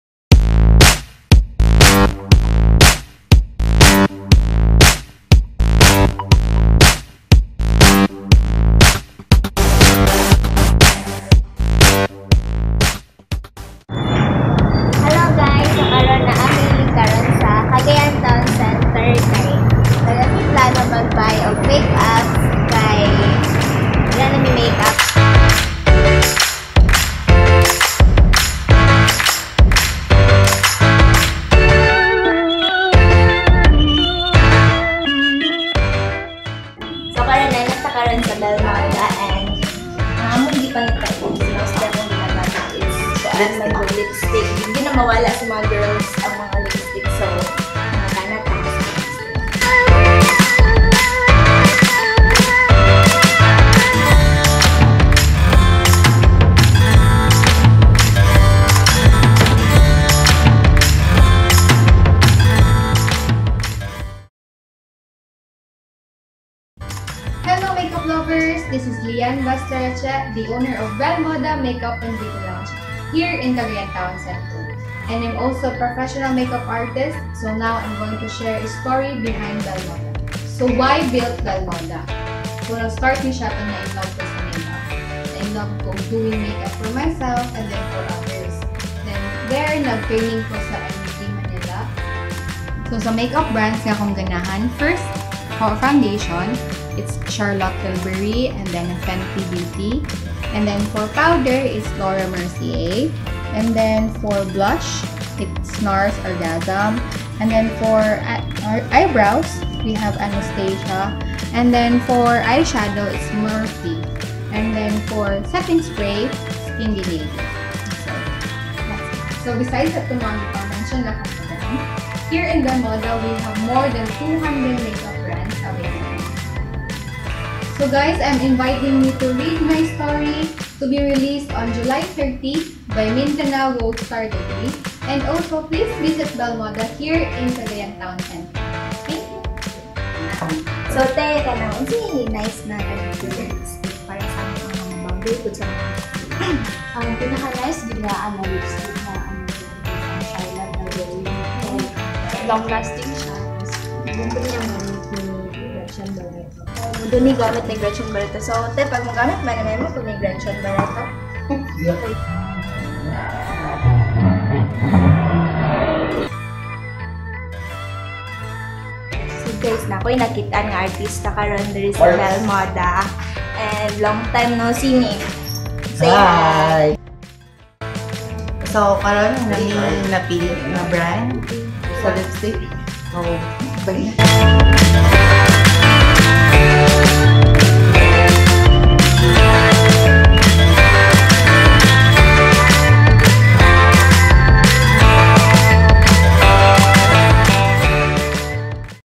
Bellemoda: From Dreams to Reality
A sneak peek of Monday's (July 30) feature release of Bellemoda Make-up and Beauty Lounge by Liane Bastareche located at Cagayan Town Center